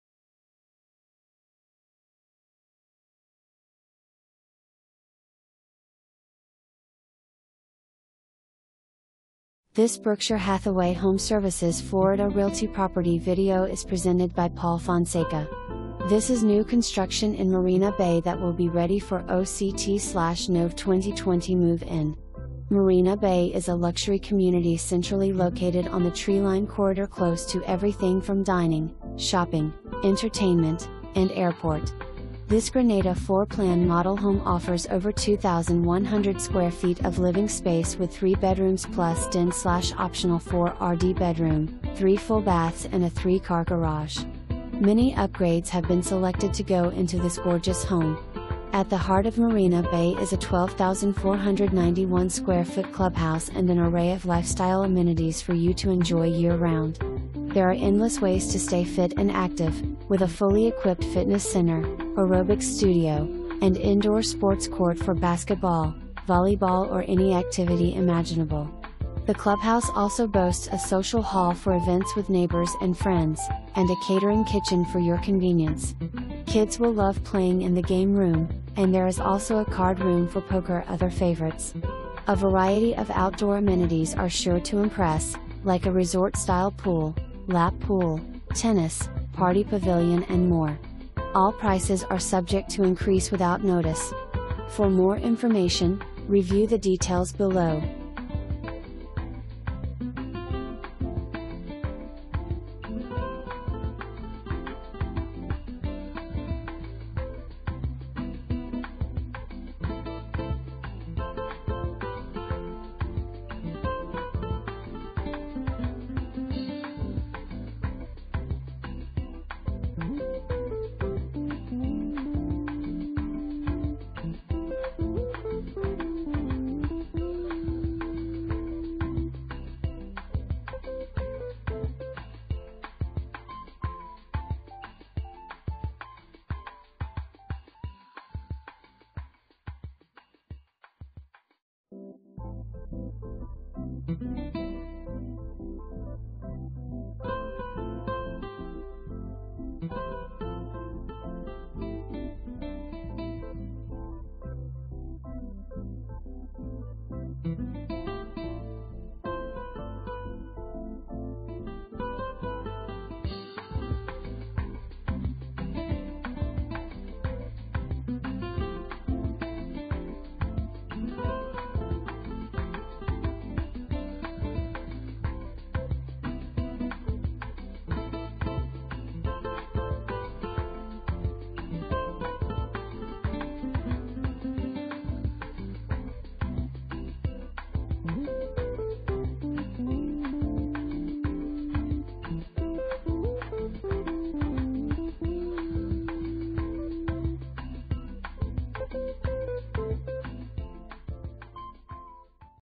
Berkshire Hathaway HomeServices Florida Realty - 10044 Chesapeake Bay DR
This is NEW CONSTRUCTION in Marina Bay that will be READY FOR OCT/NOV 2020 MOVE IN! Marina Bay is a Luxury community centrally located on the Treeline corridor close to everything from Dining, Shopping, Entertainment, and Airport! This GRENADA FLOOR PLAN MODEL HOME offers over 2,100 square feet of living space with 3 bedrooms plus Den/Optional 4rd Bedroom, 3 Full Baths and a 3 Car Garage. Many UPGRADES have been selected to go into this Gorgeous Home! At the heart of Marina Bay is a 12,491 square foot clubhouse and an array of lifestyle amenities for you to enjoy year-round. There are endless ways to stay fit and active, with a fully-equipped fitness center, aerobics studio, and indoor sports court for basketball, volleyball or any activity imaginable. The clubhouse also boasts a social hall for events with neighbors and friends, and a catering kitchen for your convenience. Kids will love playing in the game room, and there is also a card room for poker other favorites. A variety of outdoor amenities are sure to impress, like a resort-style pool, lap pool, tennis, party pavilion and more. All prices are subject to increase without notice.

Paul Fonseca
Berkshire Hathaway HomeServices Florida Realty